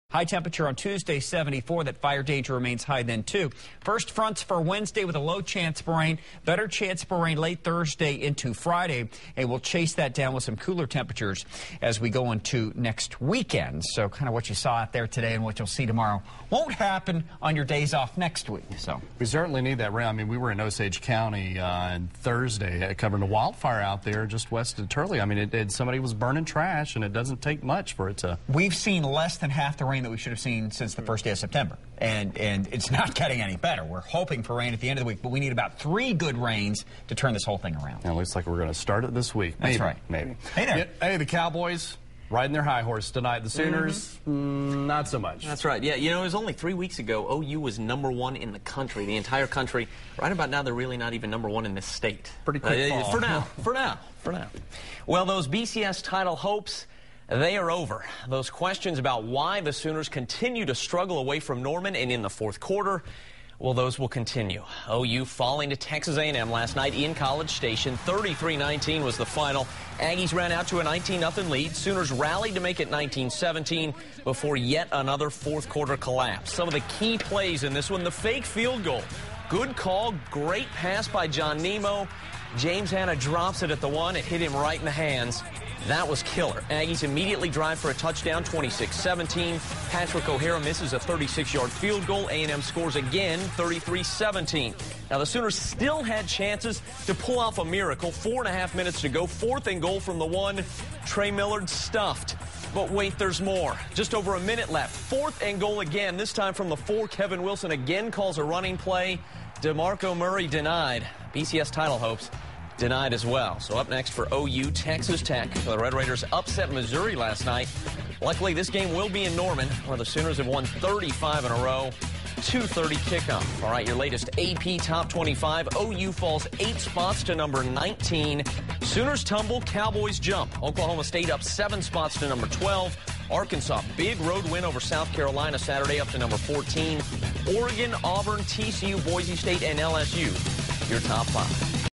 Oklahoma-Texas A&M Recap
Sooners fall to Aggies, 33-19